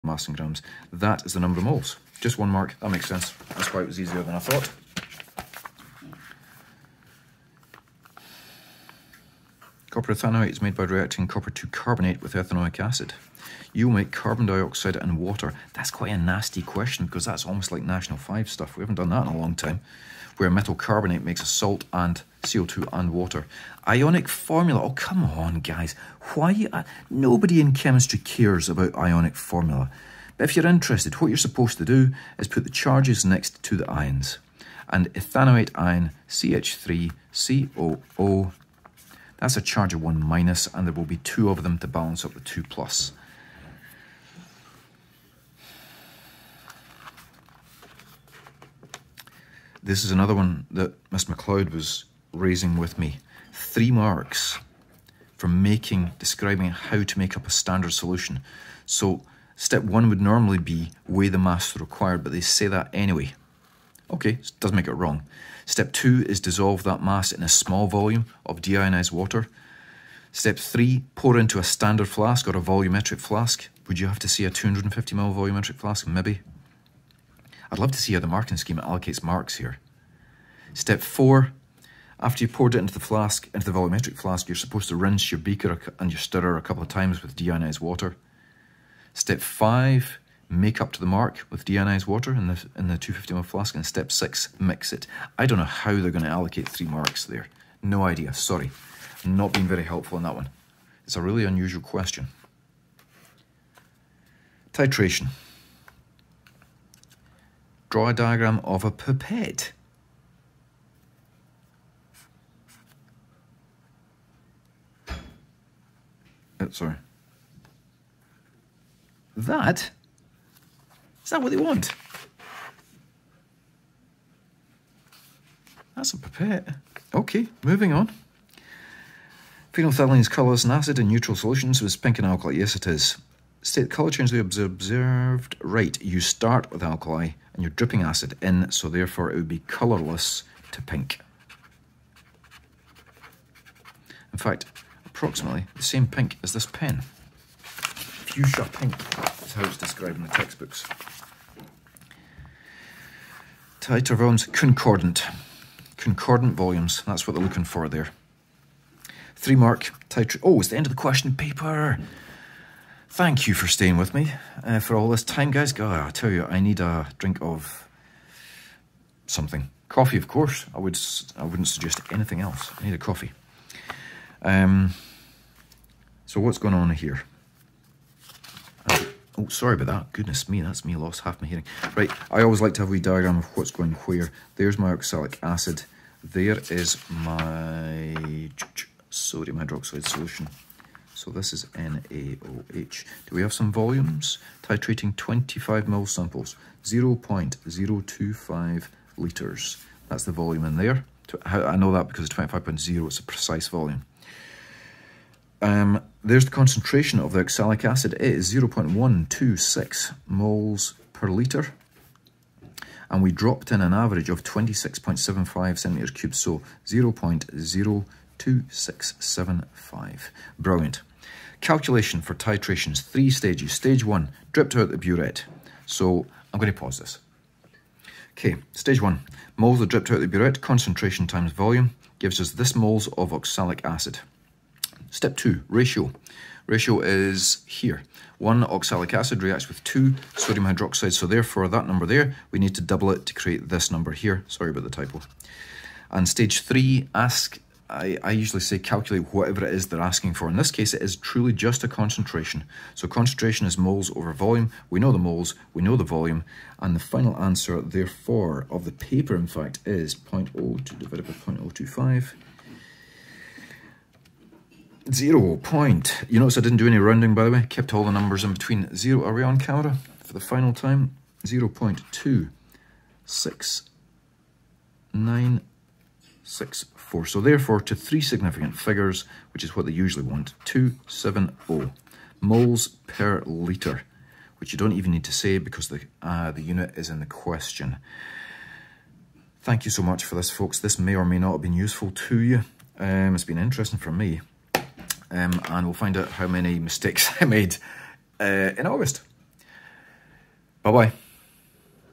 2022 Higher Chemistry Part 2 (continued)
It turns out my phone chopped the video file into two parts. Here's the remainder.
Mistake 6: Q 11. c) ii) Pink to colourless. Abi Gail for the win again.
These are for non-profit educational info only.